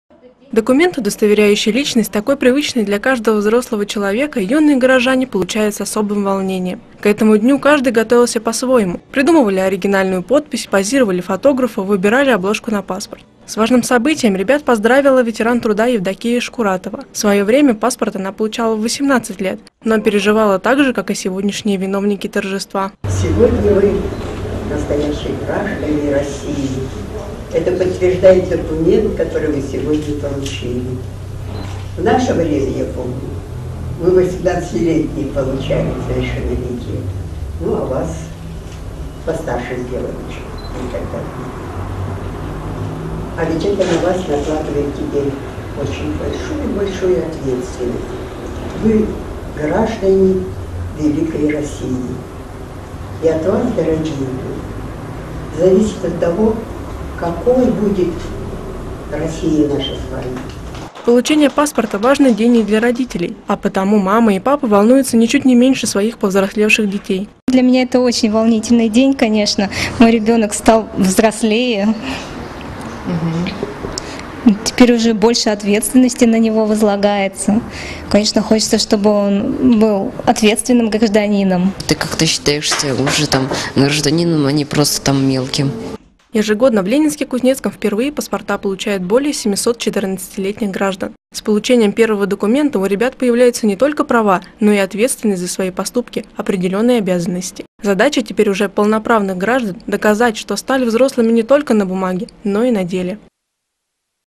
Торжественное вручение первых паспортов
В канун празднования Дня России и дня города свой первый документ – паспорт гражданина Российской Федерации - получили 11 ленинск-кузнечан. По традиции вручение прошло в торжественной обстановке в краеведческом музее.